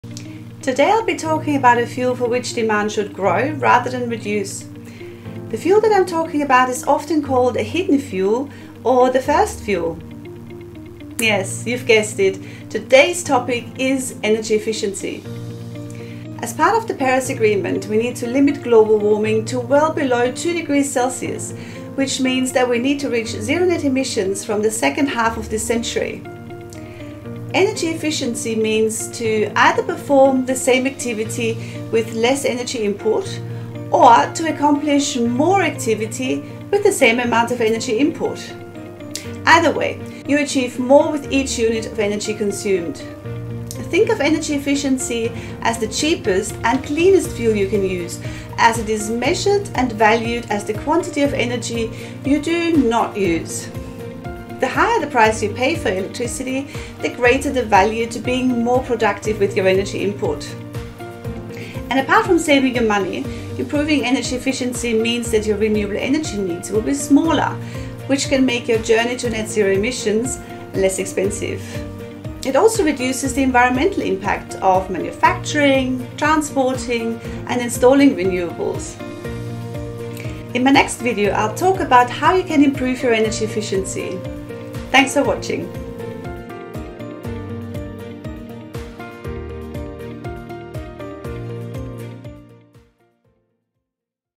The importance of energy efficiency
Today I'll be talking about a fuel for which demand should grow rather than reduce. The fuel that I'm talking about is often called a 'hidden fuel', or the 'first fuel'. Yes, you've guessed it, today's topic is energy efficiency.

As part of the Paris Agreement, we need to limit global warming to well below 2 degrees Celsius, which means that we need to reach zero net emissions from the second half of this century.

Energy efficiency means to either perform the same activity with less energy input or accomplish more activity with the same amount of energy input. Either way, you achieve more with each unit of energy consumed.

Think of energy efficiency as the cheapest and cleanest fuel you can use, as it is measured and valued as the quantity of energy you do not use. The higher the price you pay for your electricity, the greater the value to being more productive with your energy input.

Apart from saving you money, improving energy efficiency means that your renewable energy needs will be smaller, which can make your journey to net-zero emissions less expensive. It also reduces the environmental impact of manufacturing, transporting, and installing renewables.